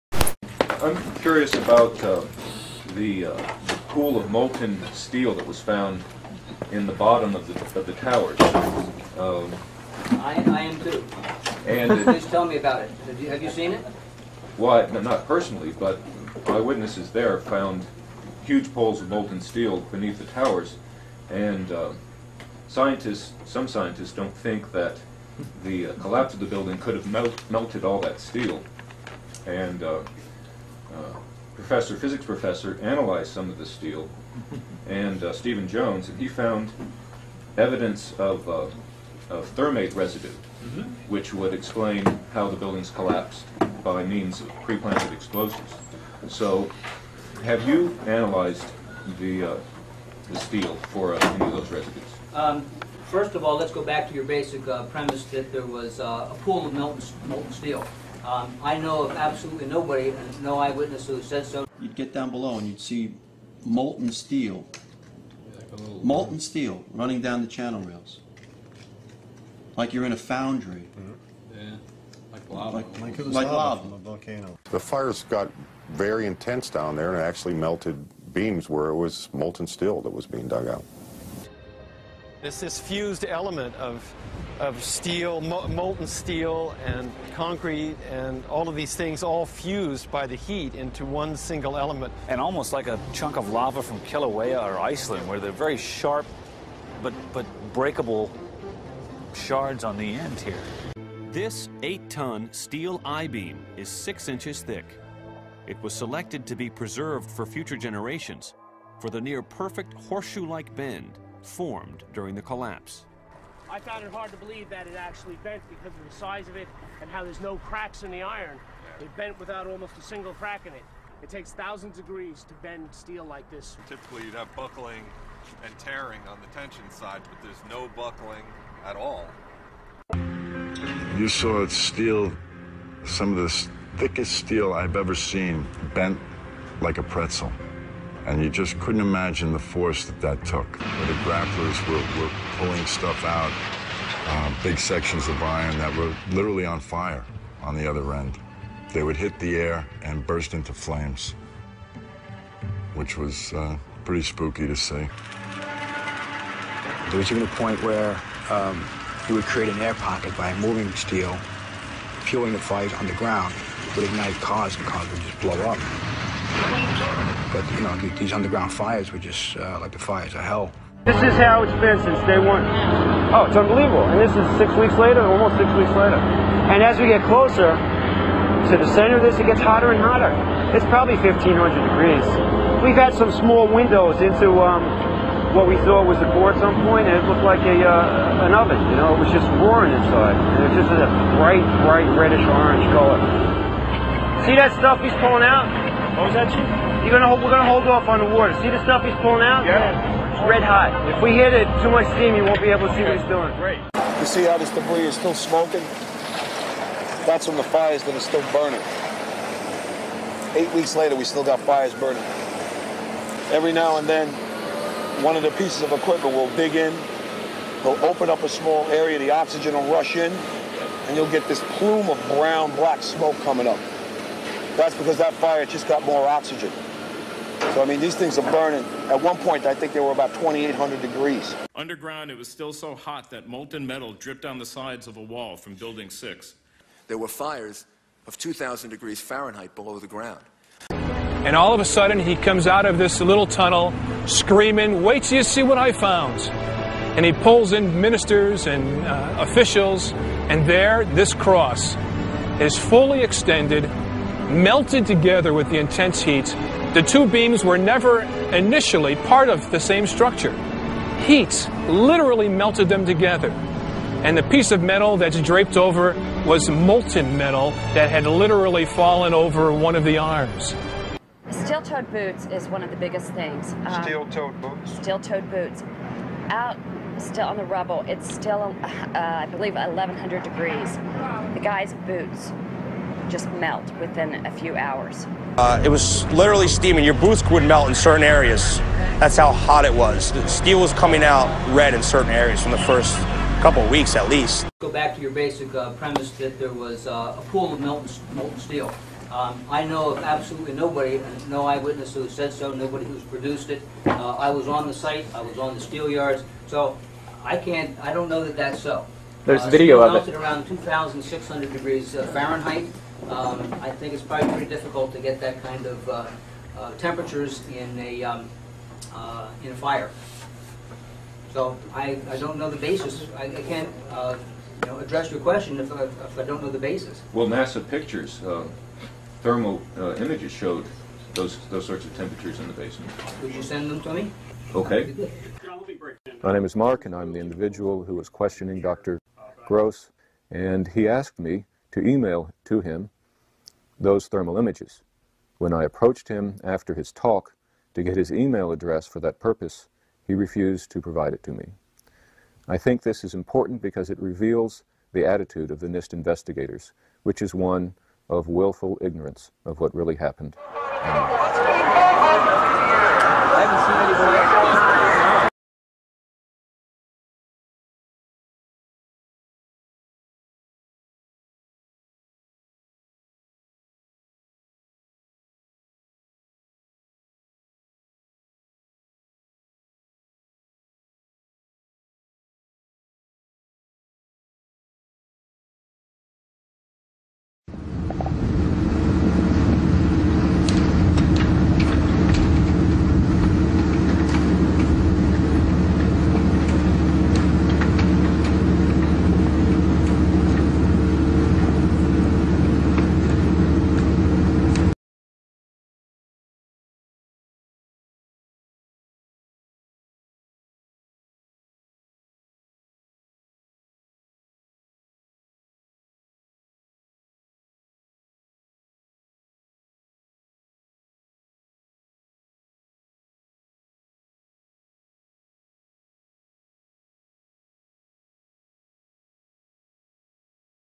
The Evidence of Molten Steel on 9/11
The scientific and video proof of the molten steel on 9/11